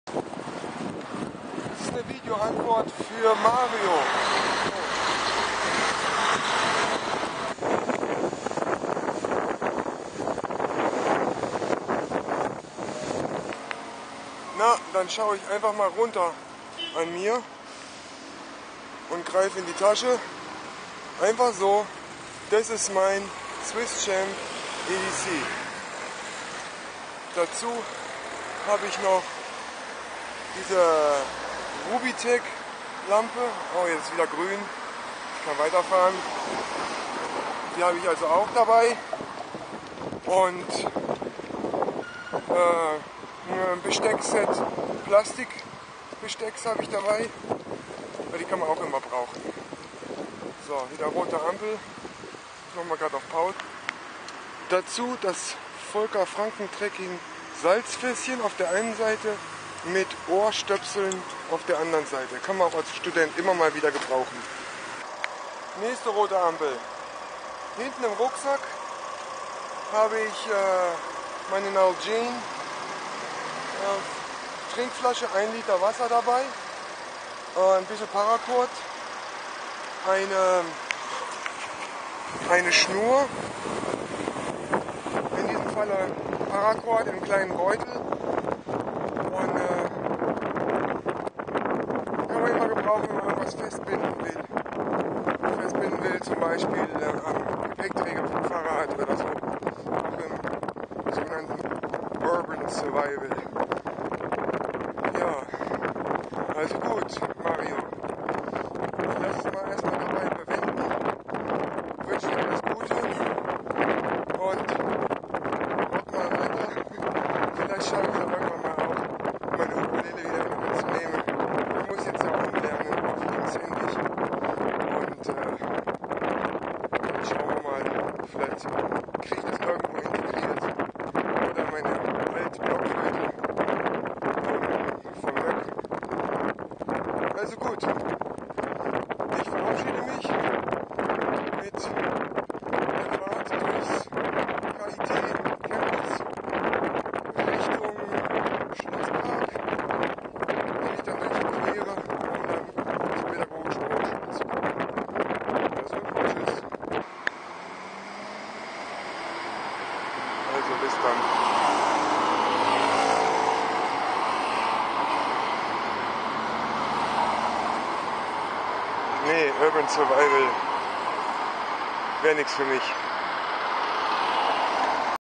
Urban EDC (VA für MsBricktop80) auf dem Weg zur Hochschule
VA für Mario (MsBricktop80)
EDC-Messer zeigen, welches wir gerade dabei haben. Ich habe eigentlich immer
- das Victorinox Swiss Champ am Mann,
- meist auch die EDC Taschenlampe / Solartaschenlampe mit Dynamokurbel
- etwas Salz und
- ein Set Ohrstöpsel
dabei.

Im Gepäck (nicht gezeigt) ein Plastik Besteckset (Löffel, Gabel), Schnur (Paracord), Kulturtäschchen (Zahnputzzeug, Deo, Mini Mikrofasertuch), meine Nalgene Trinkflasche mit 1 Liter Wasser (morgens), wo ich dann nachfüllen bei Bedarf kann.